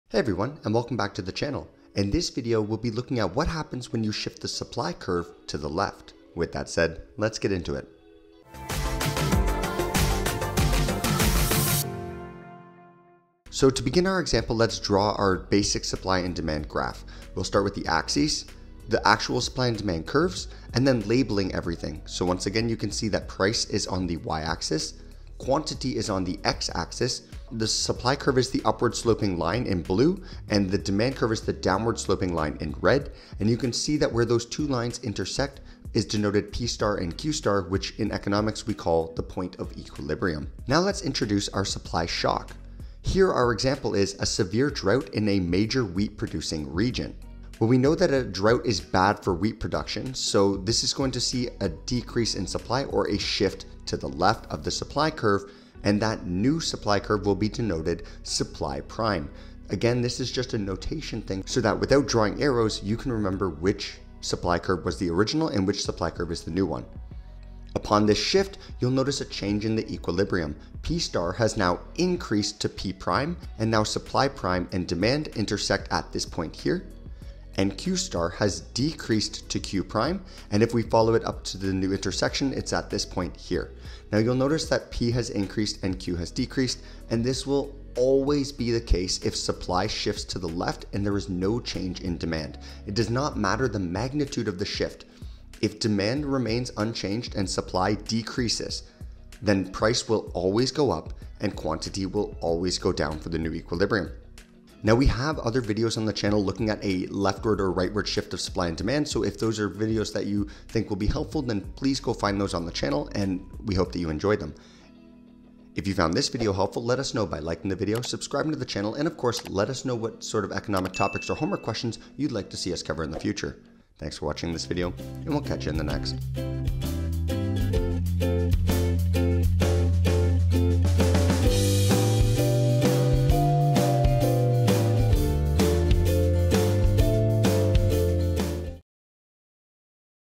Shifting the SUPPLY CURVE Leftward | Think Econ
In this video we explore what happens to equilibrium price and quantity when you shift the supply curve to the left!